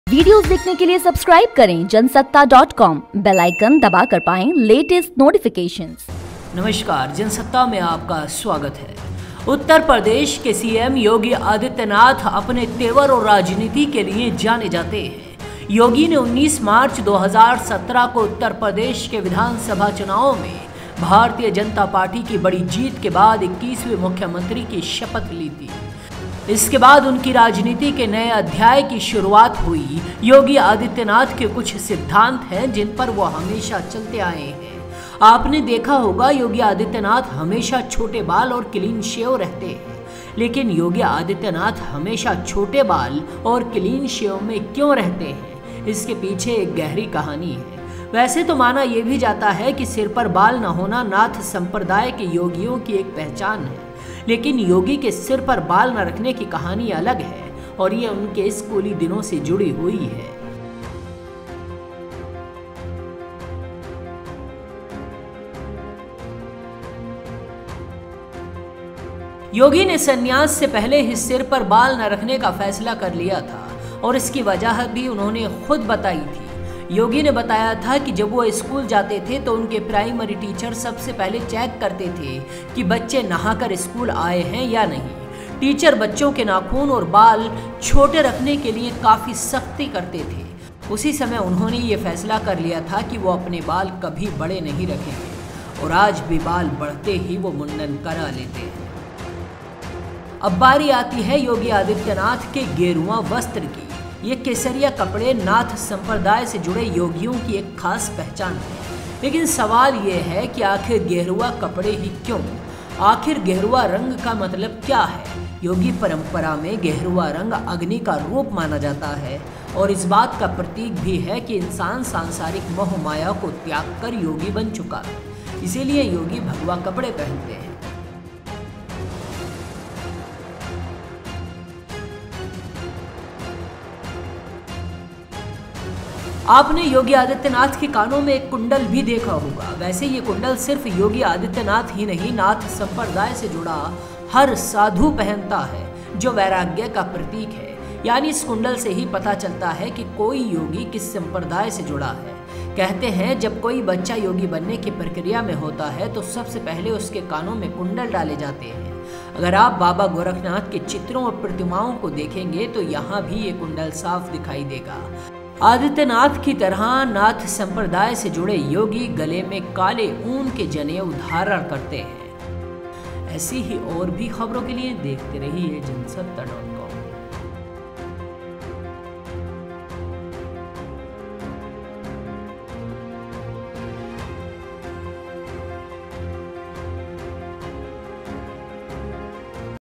तो इस वजह से योगी आदित्यनाथ ने बचपन में ही कर लिया था सिर मुंडवाने का फैसला | CM Yogi Bald Hair Cut
उत्तर प्रदेश के मुख्यमंत्री योगी आदित्यनाथ (CM Yogi) अपने भाषण और राजनीति के लिए जाने जाते है...योगी आदित्यनाथ के कुछ सिद्धांत है जिस पर वो हमेशा चलते हैं. आपने देखा होगा योगी आदित्यनाथ हमेशा छोटे बाल और क्लीन शेव रहते है... लेकिन योगी आदित्यनाथ हमेशा छोटे बाल और क्लीन शेव में क्यों रहते है. इसके पीछे एक गहरी कहानी है. और ये उनके स्कूली दिनों से जुड़ी हुई है..